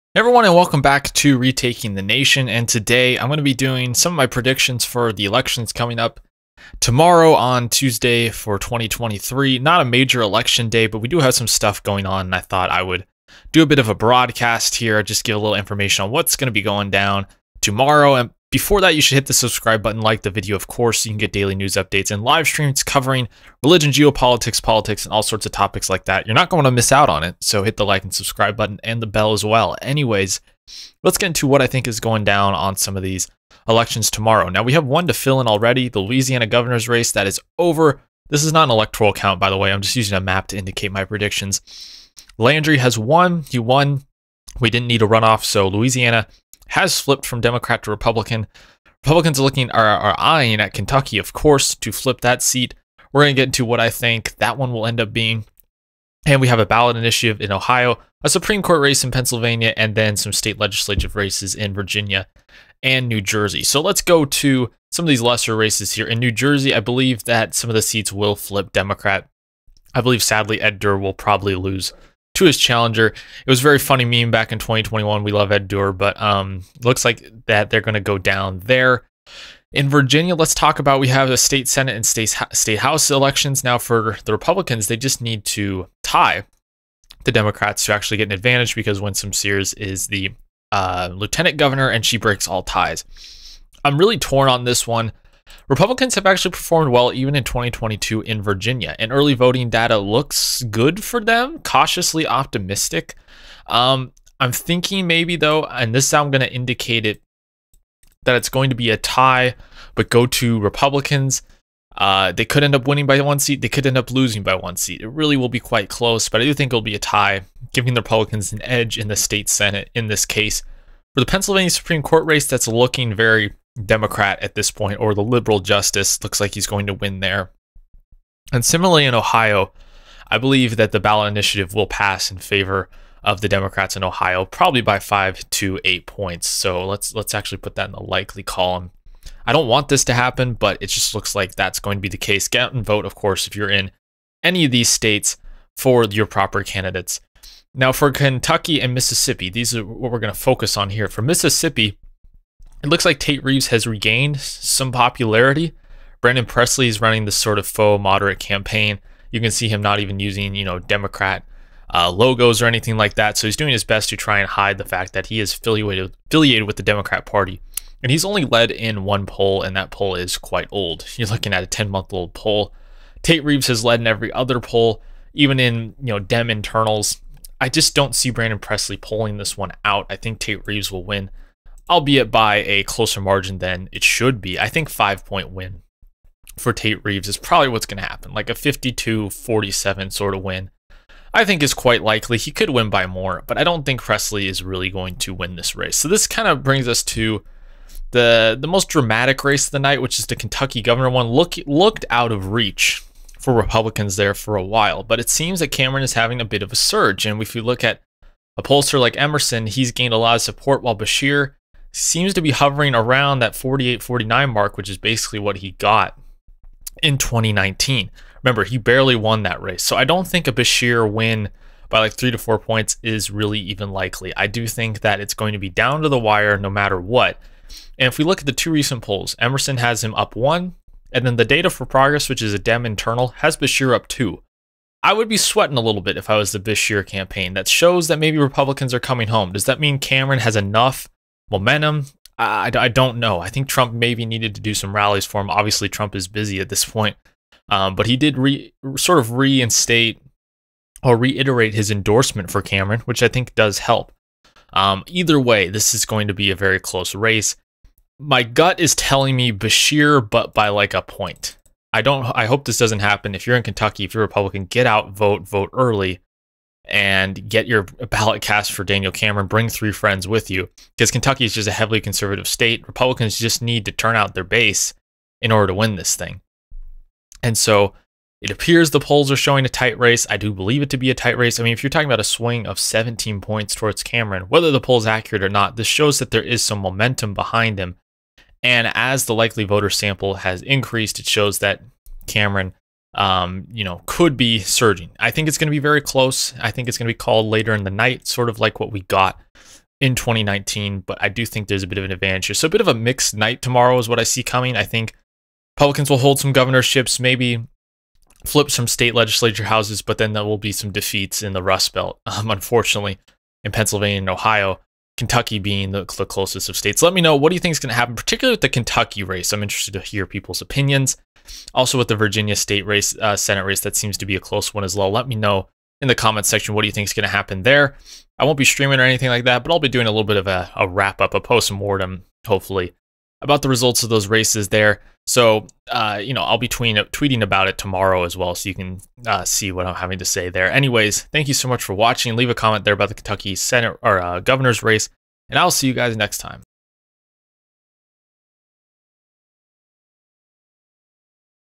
2023 ELECTION PREDICTIONS | CAN KENTUCKY TURN RED??
America First commentary, working towards a More Perfect Right Wing.